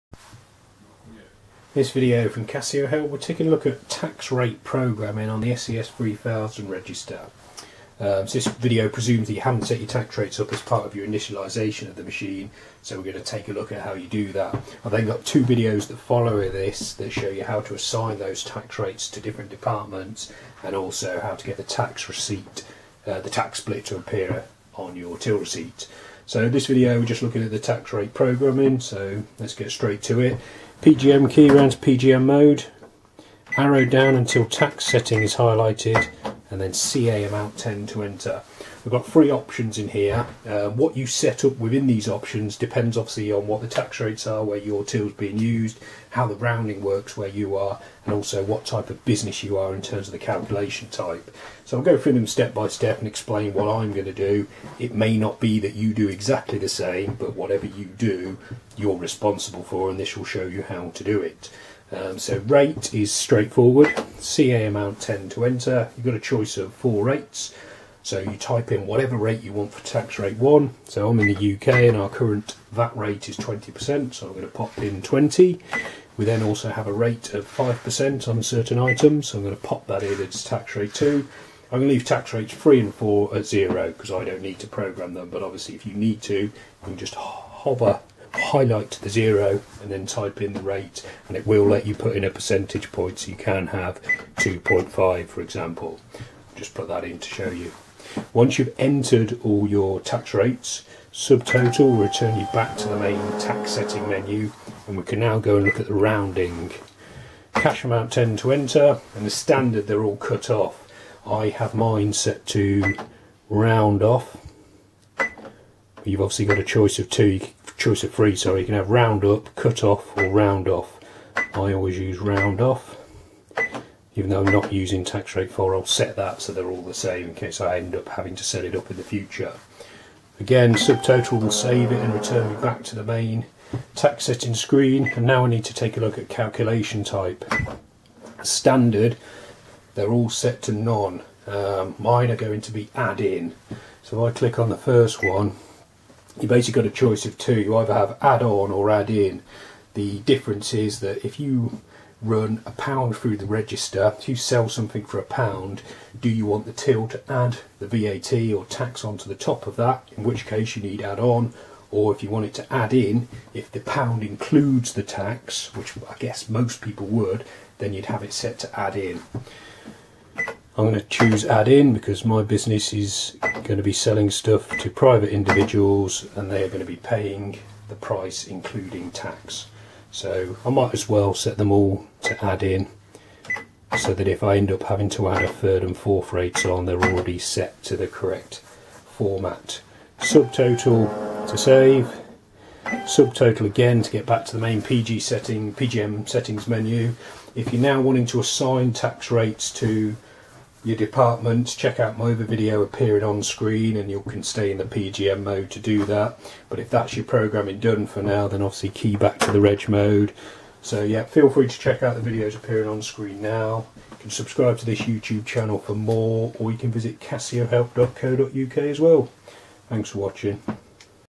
How To Program Tax Rates On The Casio SE-S3000 / PCR-T2100 / PCR-T2300 / PCR-T2400 Cash Register
How to program tax rates on the Casio SE-S3000, PCR-T2100, PCR-T2300 & PCR-T2400 cash registers.